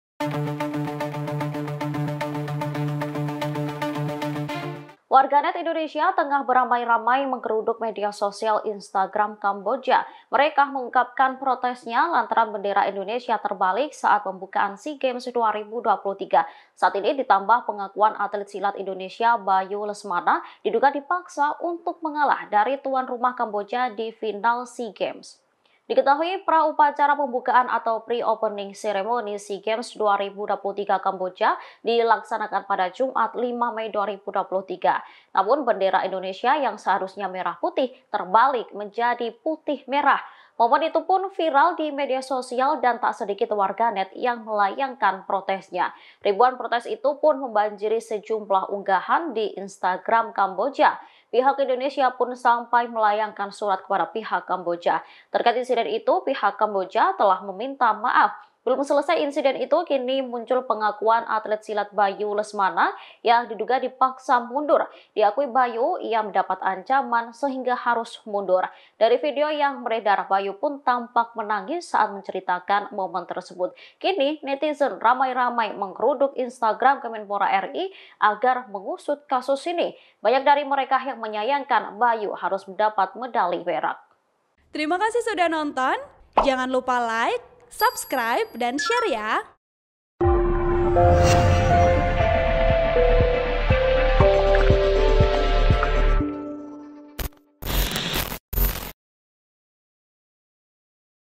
Ribuan Warganet Indonesia MERADANG, Geruduk IG Kamboja Tak Terima Ada Kecurangan & Bendera Terbalik
Mereka mengungkapkan protesnya lantaran bendera Indonesia terbalik saat pembukaan SEA Games 2023.

Saat ini ditambah pengakuan atlet silat Indonesia Bayu Lesmana diduga dipaksa untuk mengalah dari tuan rumah Kamboja di Final SEA Games.

Diketahui, pra upacara pembukaan atau pre opening ceremony SEA Games 2023 Kamboja dilaksanakan pada Jumat (5/5/2023).

Namun, bendera Indonesia yang seharusnya merah putih terbalik menjadi putih merah.

Ribuan protes itu pun membanjiri sejumlah unggahan di Instagram Kamboja.

Pihak Indonesia pun sampai melayangkan surat kepada pihak Kamboja.

Terkait insiden itu, pihak Kamboja telah meminta maaf.

Belum selesai insiden itu, kini muncul pengakuan atlet silat Bayu Lesmana yang diduga dipaksa mundur.

Diakui Bayu, ia mendapat ancaman sehingga harus mundur.

Dari video yang beredar, Bayu pun tampak menangis saat menceritakan momen tersebut.

Kini, netizen ramai-ramai menggeruduk Instagram Kemenpora RI agar mengusut kasus ini.

Banyak dari mereka yang menyanyangkan Bayu harus mendapat mendali perak.

(Tribun-Video.com/TribunPontianak.co.id)